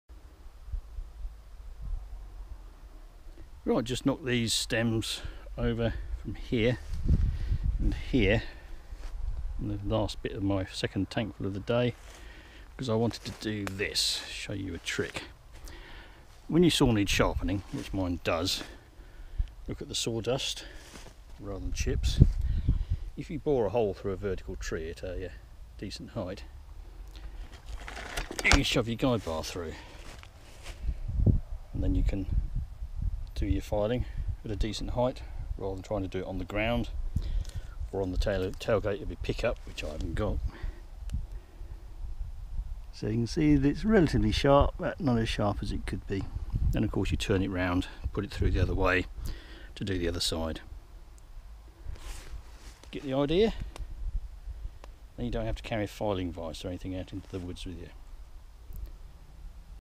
Saw chain filing 'vise' in the woods
Too grumpy to be bashing away with a blunt chain getting all sweaty and having your fells go wrong 'cos the placement of your cuts is off? Not got a truck with a tailgate you can use as a work bench? Too old to crouch over the saw and file on the ground with your knees in the mud and your back singing a descant? Try this. Works best with a longer bar and best for 'tickling' the chain, rather than correcting major damage (although you can do that).  Best plan if you're being paid is carry a spare (sharp) chain and swap them over if you hit a stone, bit of metal/the dirt.  For my firewood coppicing on this site I carry as little as possible as I'm walking a half mile up hill before I start cutting. Lack of winter access may be one reason why I have landed a 'free firewood' gig in a beautiful and diverse Ancient Woodland.

Bore in before your saw is blunt and start with the bottom quadrant of the bar tip - remember that the top quadrant is much more likely to make the saw kick back. Go for a 'tight fit' on the bar so the saw stays horizontal. You'll need to pick your stem - it needs to be upright and have enough clearance around it (why I felled the other stems). Too narrow a stem and the bar is more likely to point upwards, too fat and less bar /chain length will stick out. Reminder: mind the kickback!

This chain has done around 11 hours felling, stump cutting and cross-cutting since last sharpened in the workshop with a file. The chain is several years old and in the last quarter of its life, but will see out this season, barring steel inclusions.

Coppice stools are best finished nice and low as they tend to fall apart when cut high and the inside of the original stump rots out.  Cut low, the stump spreads over the decades - see my 'why I don't plant trees' vid. I tend to use the 'push' chain when finishing coppice stools as it pushes any dirt on the back face of the stump (which you can't see) out of the kerf to the bar tip, where it flicks off. When I've finished the cut, I tend to point the bar end at a log, and blip the throttle to get chain oil to flick off the tip taking debris with it.

Not my vid, don't know the guy, but the content is spot on and well thought through and presented.
Except for the bit about 'rakers'. Those 'double rakers' are a depth gauge (which limits the cut depth of it's companion cutter to the most efficient depth) and an anti-kickback ramp that limits kickback from the chain. Pro chains tend not to have those. You get rakers on bowsaws where they clear sawdust from the kerf and in fish gills where they keep gills clean and/or harvest organic particles from the water as food. I guess that's 'linguistic shift', folks, but it makes understanding harder. If you are new to sharpening, watch Tinman's vid, think about it, watch it again and then try it.